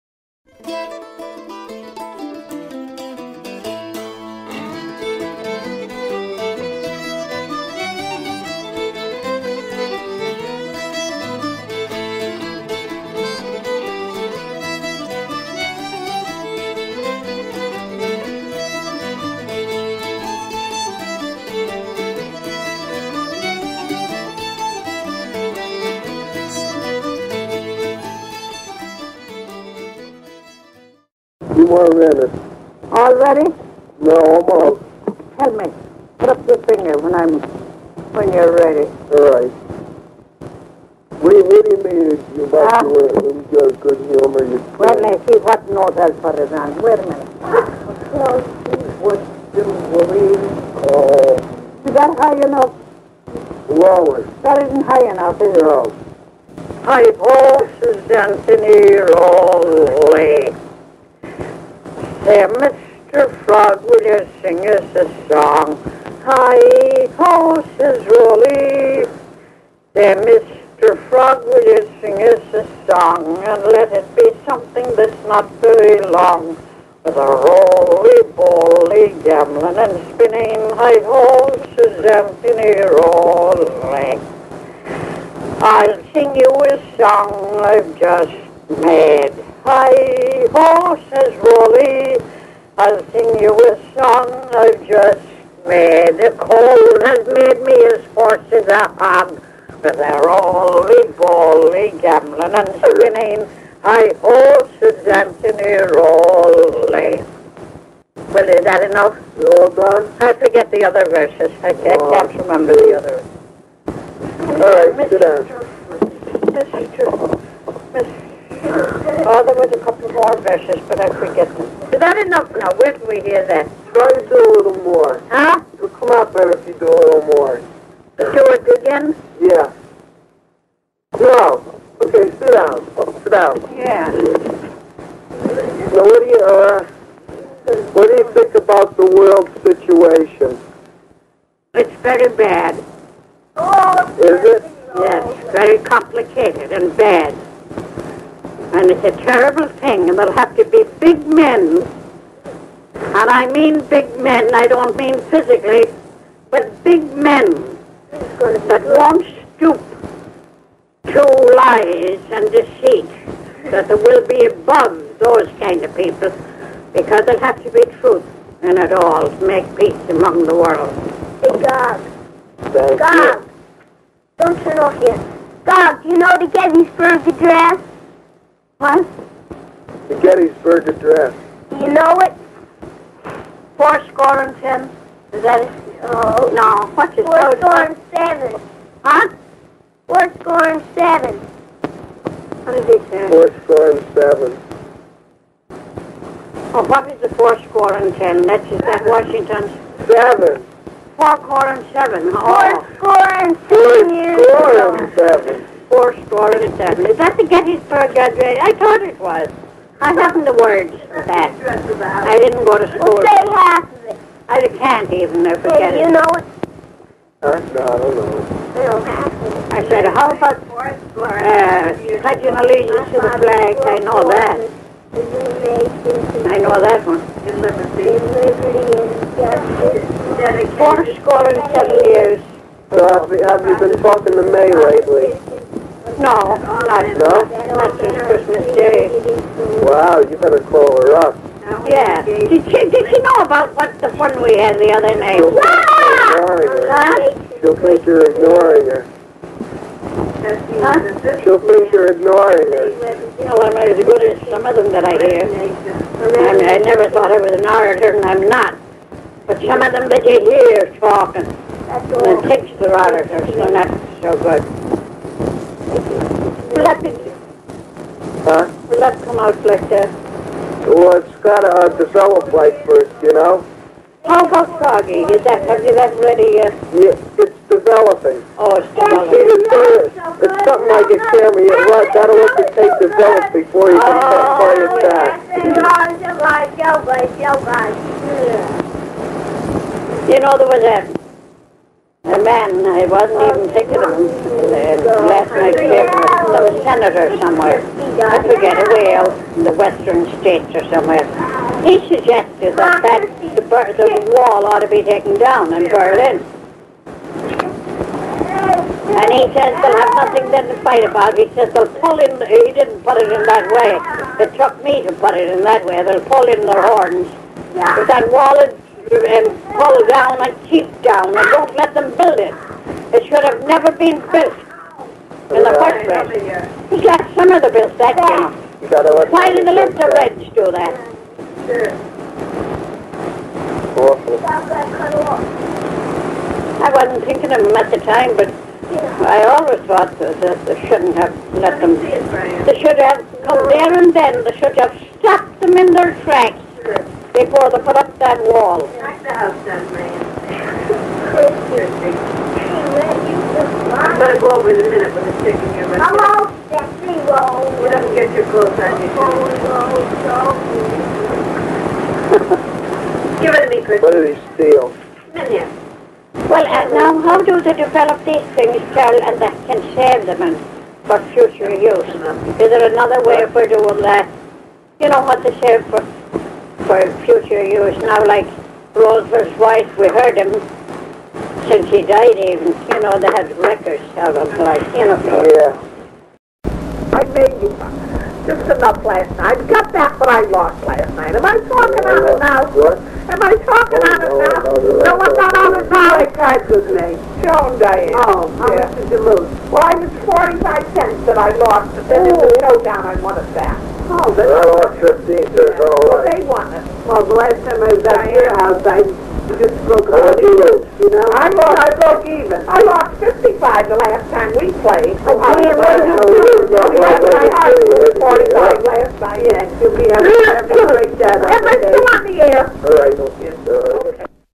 My Grandmother - Catherine (Flood) Gunther
Updated 12/1/2013 - Video of snapshots and audio of my Grandmother affectionately nicknamed Gaga.